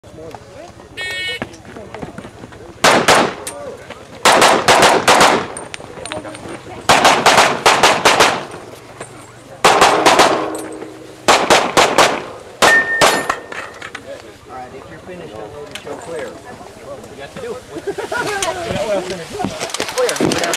Double Tap 2009 stage 6 Steven Zerwas
2nd Master on this stage.  There was slight pause after I shot the first port...I forgot what I was supposed to do.  I think I needed more caffine.